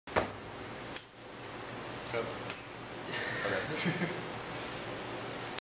At Five Towns College Working on my Movie
Not much footage, just showing the camera i was using to record my movie. Called "The Essay" If i can after i make it, i'll upload it to my channel.